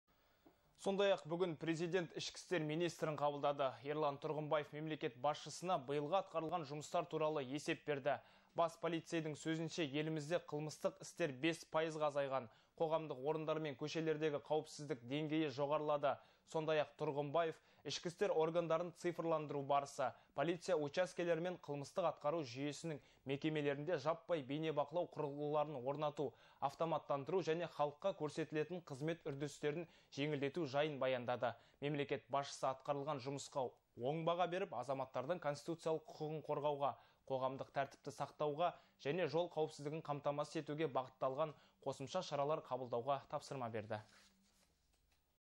Мемлекет басшысы ішкі істер министрі Ерлан Тұрғымбаевты қабылдады
Сондай ақ бүгін президент Ішкі істер министрін қабылдады. Ерлан Тұрғымбаев мемлекет  басшысына биылғы атқарылған жұмыстар туралы есеп берді. Бас полицейдің сөзінше елімізде қылмыстық істер 5 пайызға азайған. Қоғамдық орындар мен көшелердегі қауіпсіздік деңгейі жоғарылады. Сондай ақ Тұрғымбаев ішкі істер органдарын цифрландыру барысы, полиция учаскелері мен қылмыстық-атқару жүйесінің мекемелерінде жаппай бейнебақылау құрылғыларын орнату, автоматтандыру және халыққа көрсетілетін қызмет үдерістерін жеңілдету жайын баяндады. Мемлекет басшысы атқарылған жұмысқа оң баға беріп, азаматтардың конституциялық құқығын қорғауға, қоғамдық тәртіпті сақтауға және жол қауіпсіздігін қамтамасыз етуге бағытталған қосымша шаралар қабылдауға тапсырма берді.